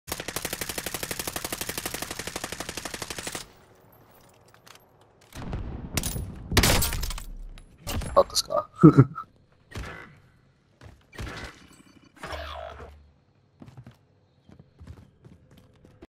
Lucky Headshot with the 3-line Rifle
Just pointed randomly into the sky and took the shot.